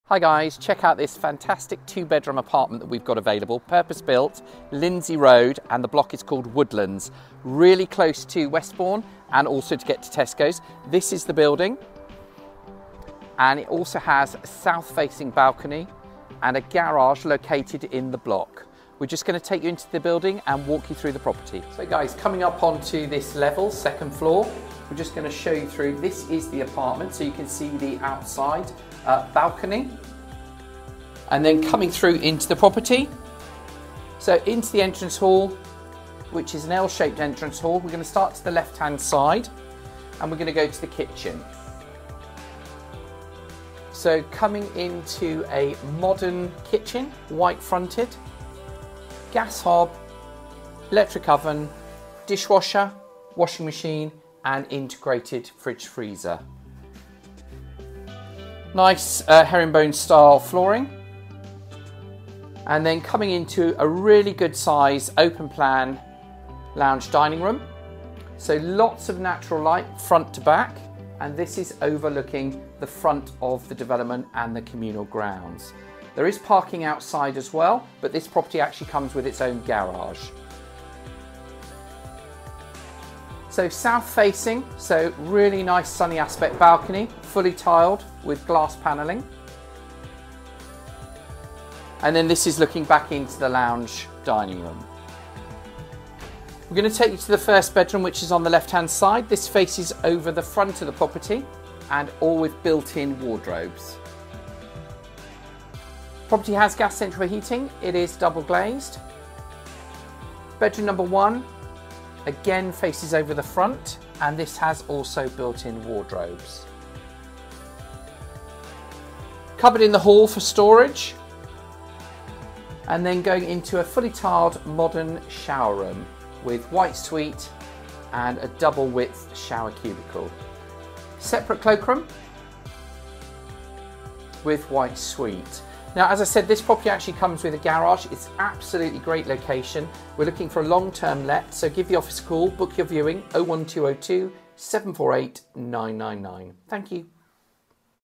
Two Bedroom Apartment To Rent in Branksome Park / Thacker and Revitt
SOUTH FACING IN LYNDSAY ROAD, POOLE, £1200PCM. Take a look at this spacious purpose built apartment located in Branksome Park close to Tesco and railway station. This apartment is being unfurnished and is on a min 6 month let. There is a modern shower room and seprate cloakroom. Modern white fitted kitchen with integrated dishwasher, oven and hob and extractor. Open plan lounge dining room with patio doors leading onto a tiled and glass panelled sunny aspect balcony overlooking the communal gardens. Two double bedrooms both with built in wardrobes. There is also a single garage allocated with the property. This block is within walking distance of popular Westbourne for shops , bars and coffee shops. Just a stones throw away from Tesco Store in Branksome. Easy access to the beach. This will be an ideal let for a single or proff couple. Council Tax Band C. Deposit £1300.00. Avail February 2024.